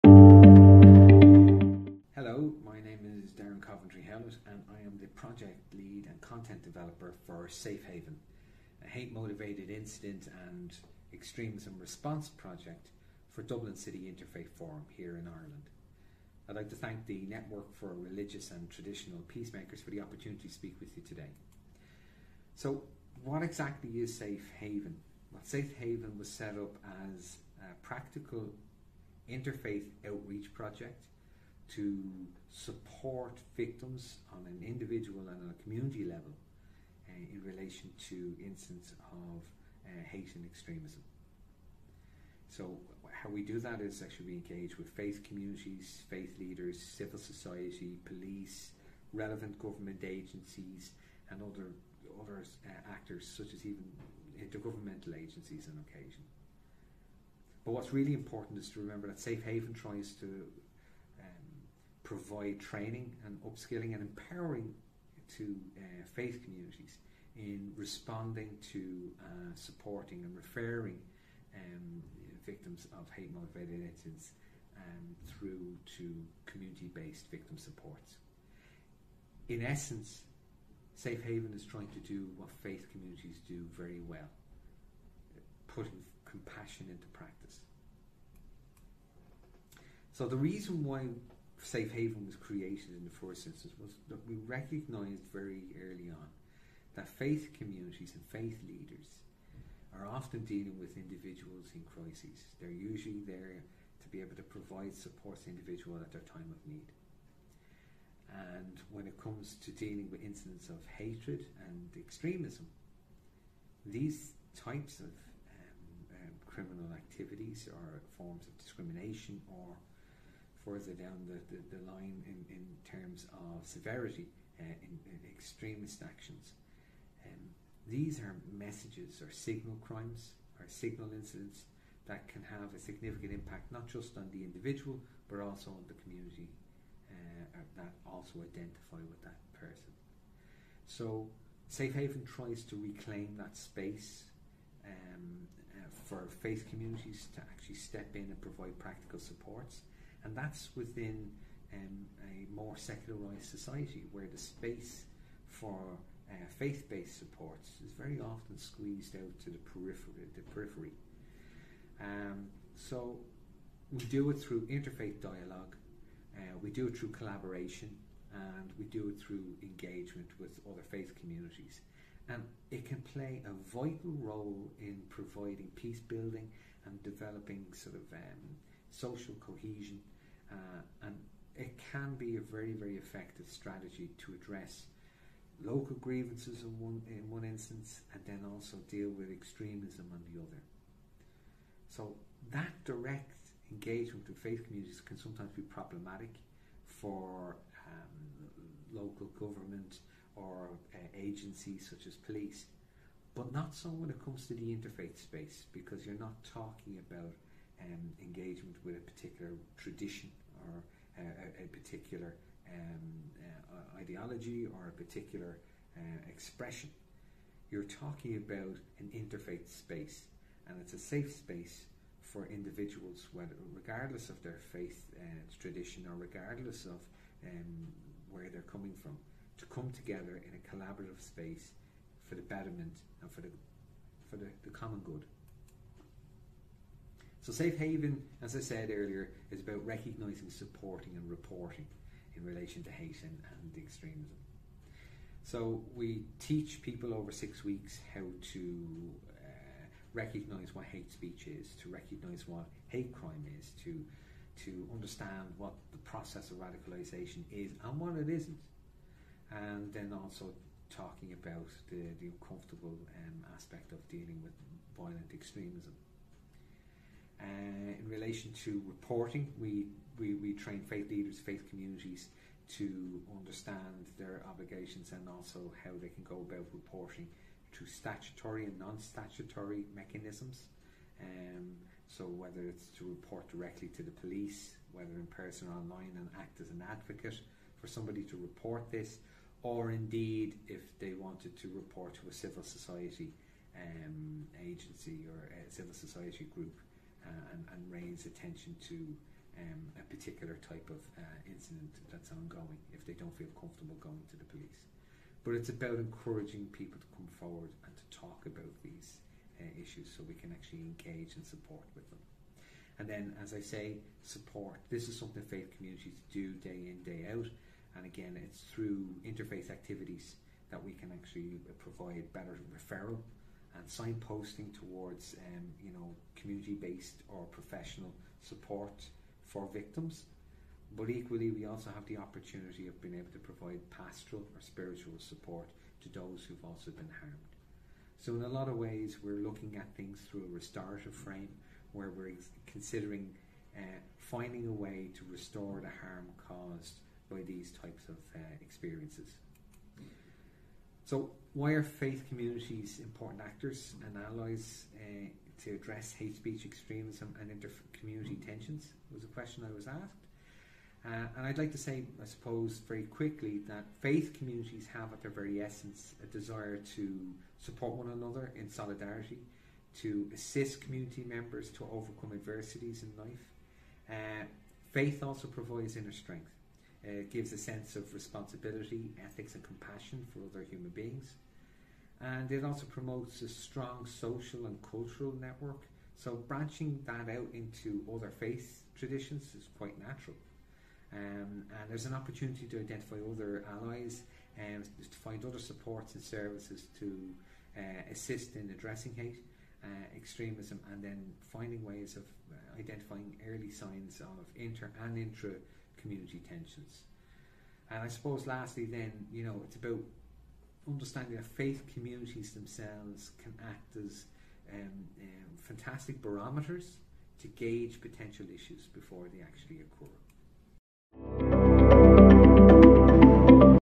PAVE Project: Faith Leaders and Addressing Hate Speech, Extremism and Inter-Community Tensions
As part of the European Union funded “Preventing and Addressing Violent Extremism through Community Resistance (PAVE)” Project, Darren Coventry-Howlett from Safe Haven shares why faith communities are important actors and allies to address to address hate speech, extremism and inter-community tensions.

The European research project PAVE aims to examine driving and mitigating factors of radicalisation and violent extremism in the Balkans and MENA region.